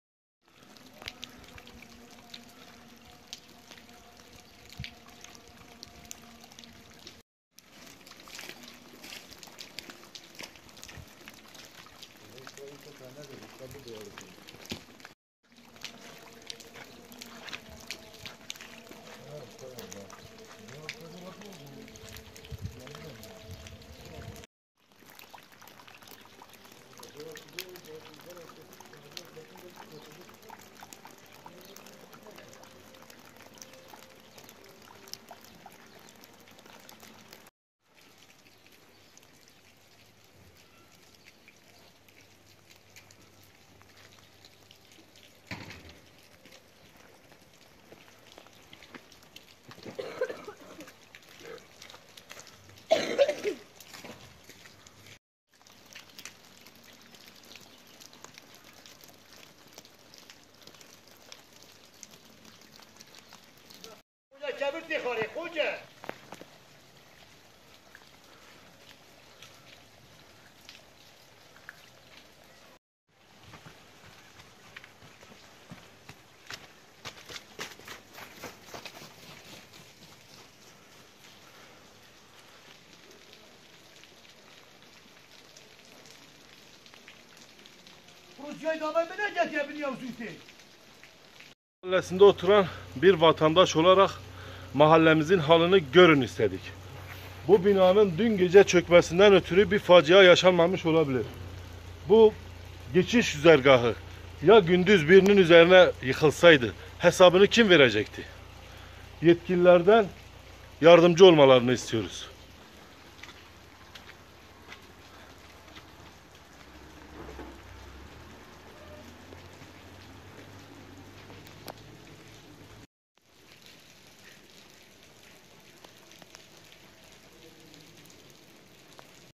Metruk binalar vatandaşları tedirgin ediyor
Bitlis'te aşırı yağışlar nedeniyle bir bir yıkılan metruk binalar, vatandaşların korkulu rüyası haline geldi.

Değerli İzleyicilerimiz;

TÜKÇE

KÜRTÇE

ARAPÇA

İNGİLİZCE

FARSÇA

SORANİ